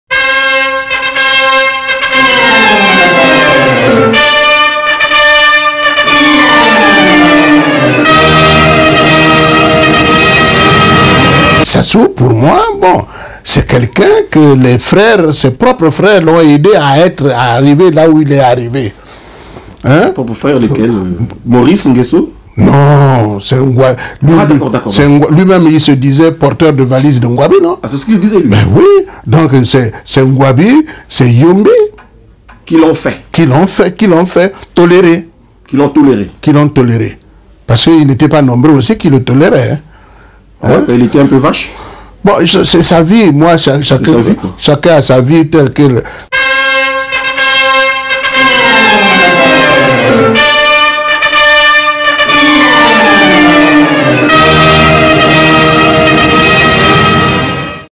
Sassou a tout d'un mauvais camarade !
A lui seul, ce ngaya a pourri la vie de plusieurs congolais émérites et détruit la nation.